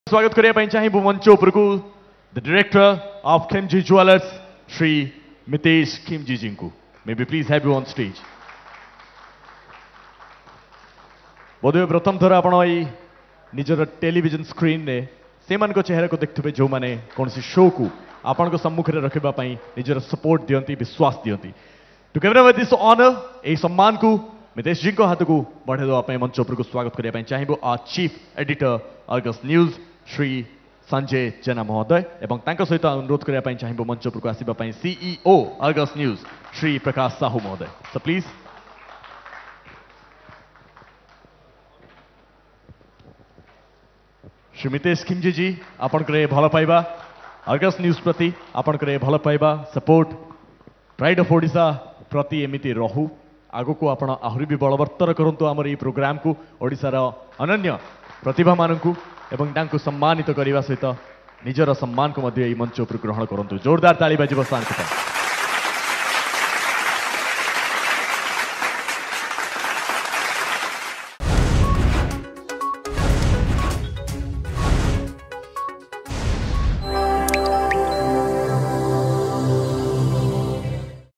Pride of Odisha | Mitesh Khimji felicitated by Argus CEO Prakash Sahu & Editor-in-Chief Sanjay Jena
Khimji Jewellers Director Mitesh Khimji felicitated by Argus News CEO Prakash Sahu and Editor-in-Chief Sanjay Jena.